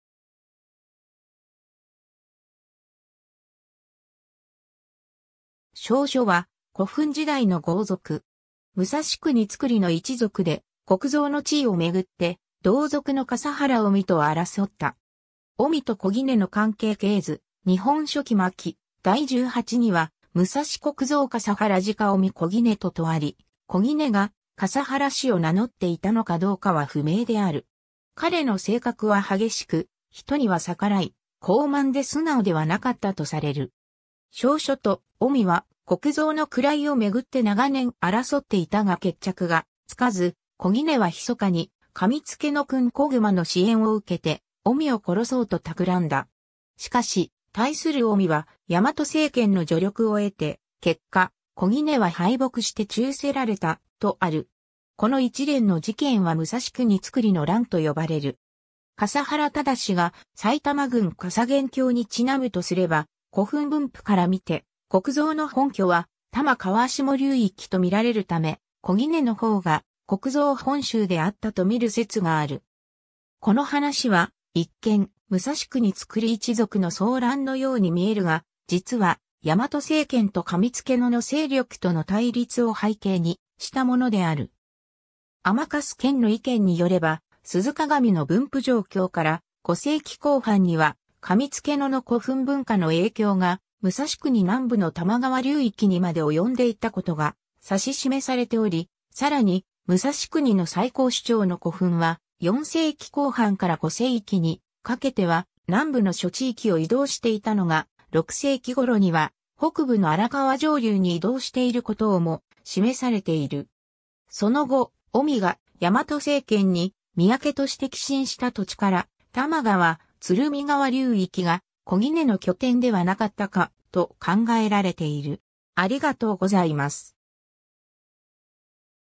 小杵
小杵（おき/おぎ、生年不明 - 安閑天皇元年閏12月（推定534年））は、古墳時代の豪族。
武蔵国造の一族で、国造の地位を巡って同族の笠原使主と争った。
 使主と小杵の関係系図 （『諸系譜』による） 『日本書紀』巻第十八には「武蔵国造（むさし の くにのみやつこ）笠原直使主（かさはら の あたひ おみ)と同族（うがら）小杵（をき）と」とあり、小杵が笠原氏を名乗っていたのかどうかは不明である。
彼の性格は激しく、人には逆らい、高慢で素直ではなかったとされる。
 小杵と使主は国造の位を巡って長年争っていたが決着がつかず、小杵はひそかに上毛野君小熊の支援を受けて使主を殺そうと企んだ。
しかし、対する使主は大和政権の助力を得て、結果、小杵は敗北して誅せられた、とある。
この一連の事件は武蔵国造の乱と呼ばれる。
 笠原直が埼玉郡笠原郷に因むとすれば、古墳分布から見て国造の本拠は多摩川下流域と見られるため、小杵の方が国造本宗であったと見る説がある。
 この話は一見、武蔵国造一族の争乱のように見えるが、実は大和政権と上毛野の勢力との対立を背景にしたものである。
甘粕健の意見によれば、鈴鏡の分布状況から、5世紀後半には上毛野（上野）の古墳文化の影響が武蔵国南部の多摩川流域にまで及んでいたことが指し示されており、さらに、武蔵国の最高首長の古墳は、4世紀後半から5世紀にかけては南部の諸地域を移動していたのが、6世紀頃には北部の荒川上流（埼玉県行田市の埼玉古墳群あたり）に移動していることをも示されている。
その後、使主が大和政権に屯倉として寄進した土地から、多摩川・鶴見川流域が小杵の拠点ではなかったか、と考えられている。